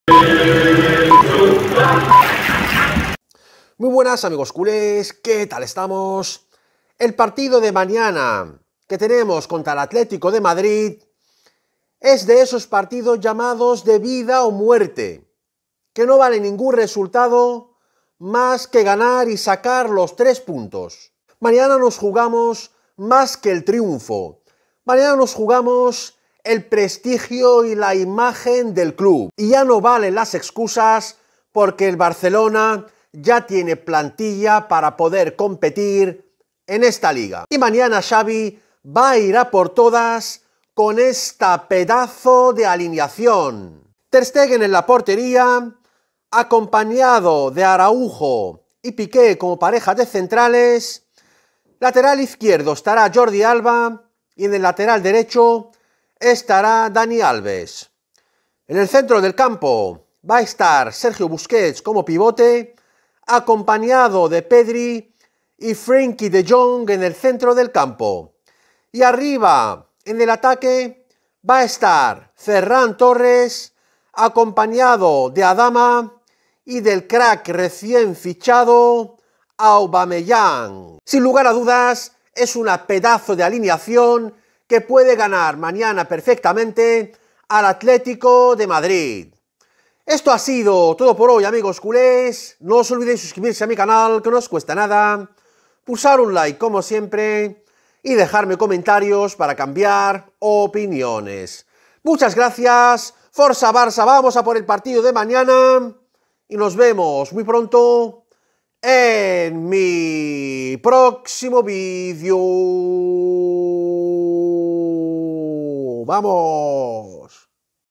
BOOM: SE CONFIRMA LA BRUTAL ALINEACIÓN DE XAVI CONTRA EL ATLÉTICO | BARCELONA VS ATLÉTICO DE MADRID
Se acaba de confirmar el brutal once titular del Barcelona de Xavi contra el Atlético de Madrid